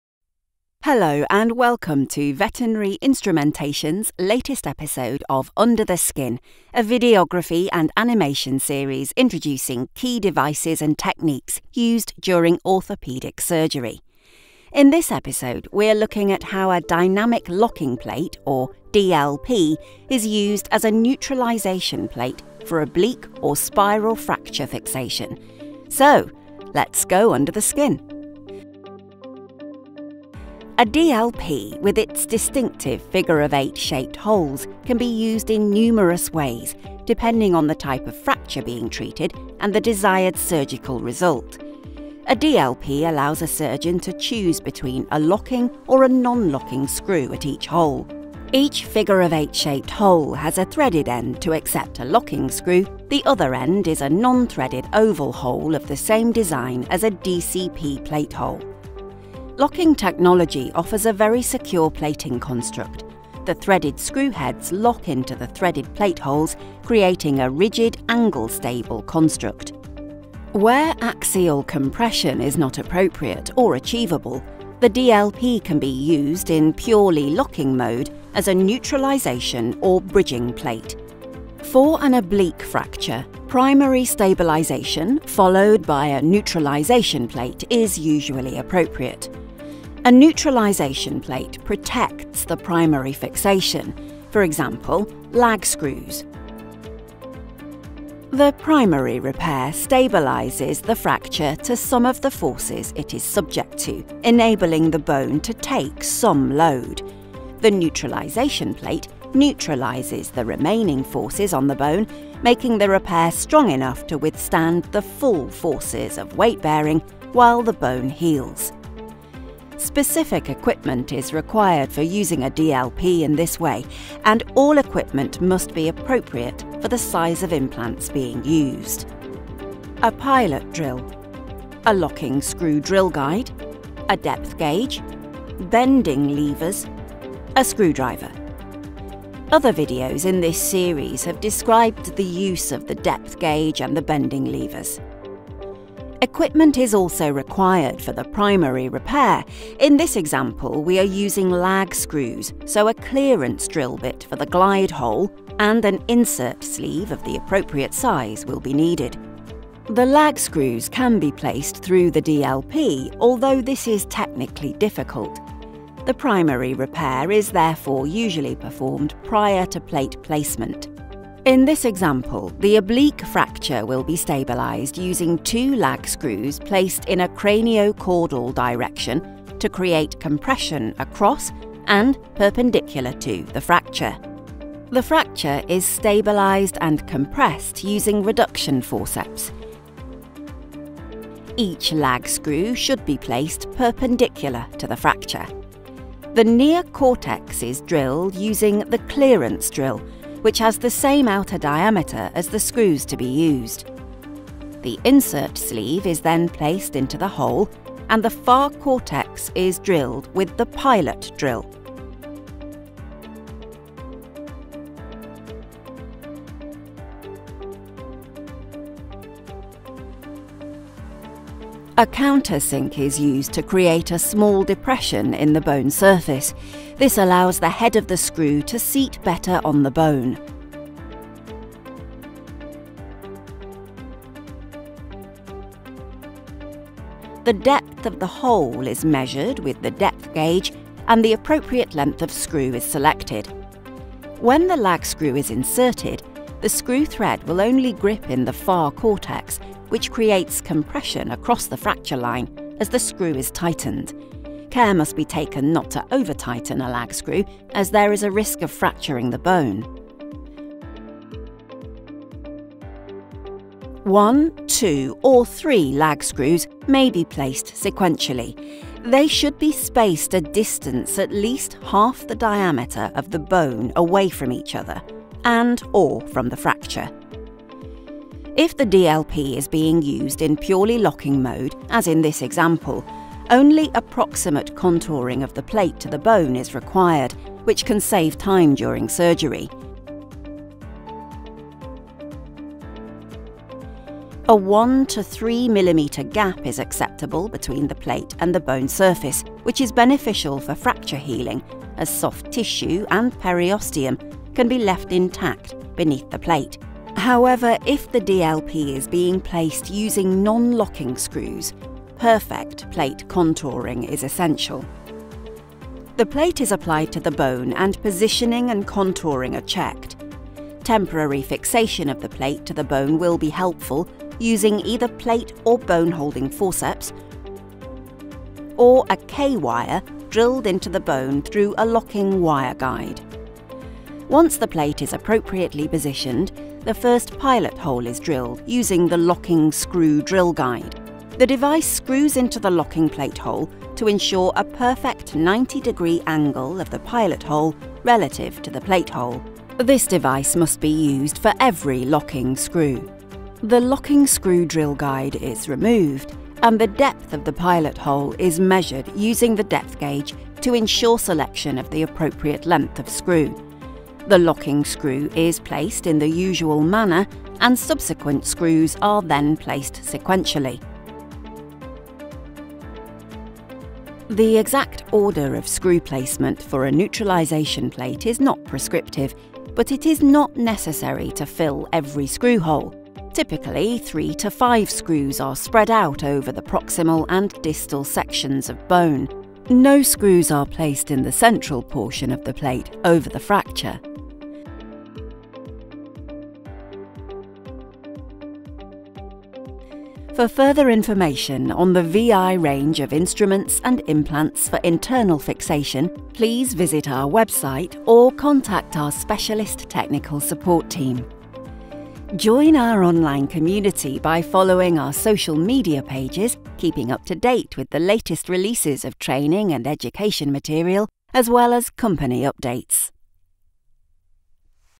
Locking Plate Used in Neutralisation Mode (Veterinary Instrumentation Orthopaedic Technique Insight)
In our latest episode of our Under The Skin series, we focus on the locking plate being used in neutralisation mode.

This video was created with leading veterinary Orthopaedic Specialist, Gareth Arthurs. In this episode, we continue our series of plating demonstrations using the Dynamic Locking Plate (DLP). A DLP may be utilised in several different ways, with the ability to combine the strength and stability of the locking construct with axial compression using non-locking cortical screws, where clinically appropriate.

In this video, we demonstrate plating of a transverse fracture using a Dynamic Locking Plate as a neutralisation plate in conjunction with lag screws.